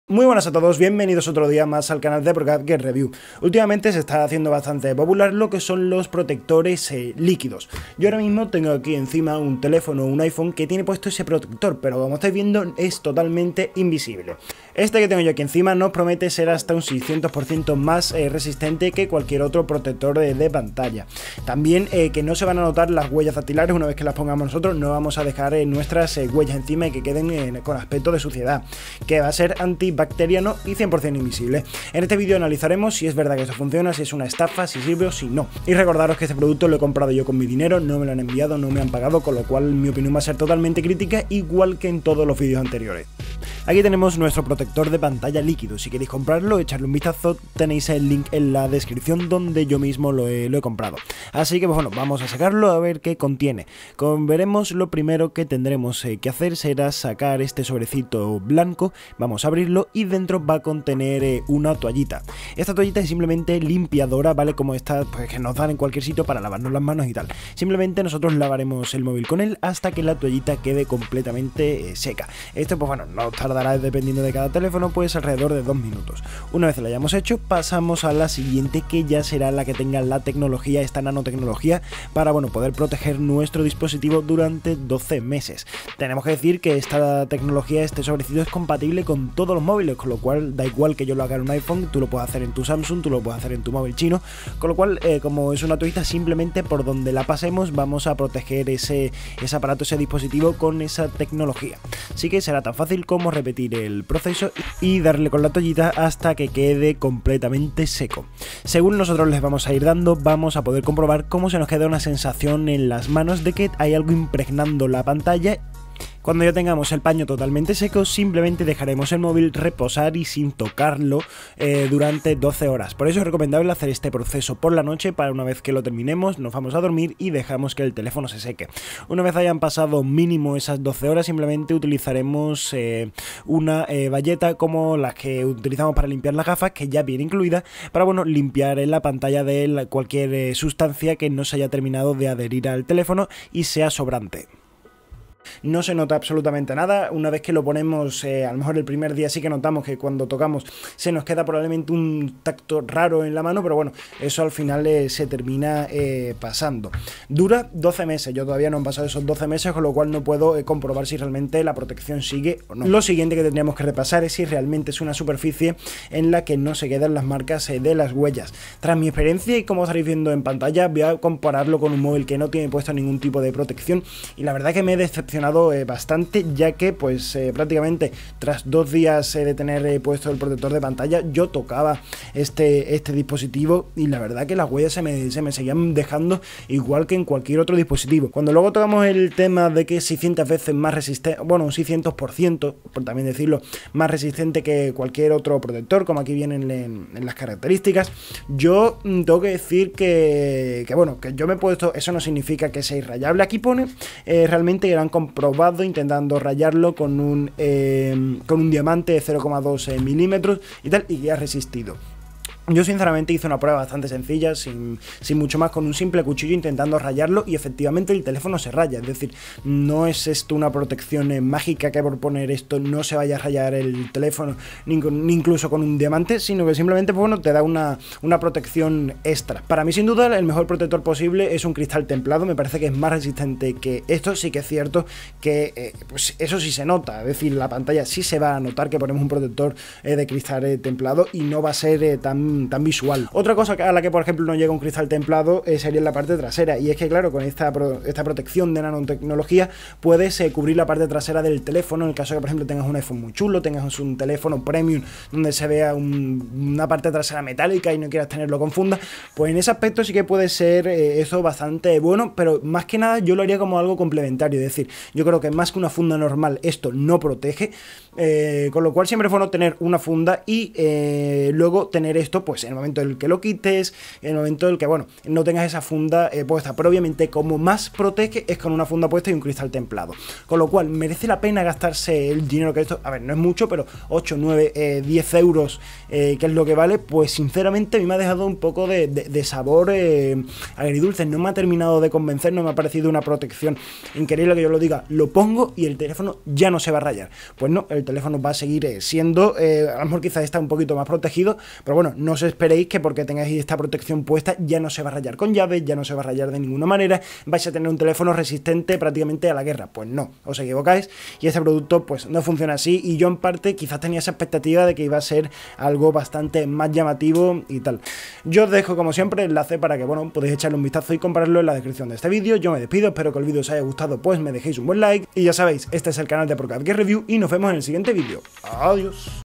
Protector de Pantalla Líquida ¿Estafa o Funciona?
COMPRA Protector de Pantalla Líquida
Es universal, sirve para todos los smartphones.

Dice ser:  ha sido diseñado para proteger todo tipo de dispositivos multimedia como teléfonos móviles, tablets, cámaras, ordenadores portátiles, televisores y sistemas de navegación por satélite.Se trata de un producto con destacados beneficios ya que es antibacteriano, anti-abrasión, resistente al agua, reduce las huellas digitales y tiene una vida útil de hasta 12 meses.
El revestimiento de cristal líquido, flexible y transpirable, es de aproximadamente 100 nanómetros de grosor, que es 500 veces más delgadas que un cabello humano, y por lo que es completamente indetectable a pesar de sus beneficios.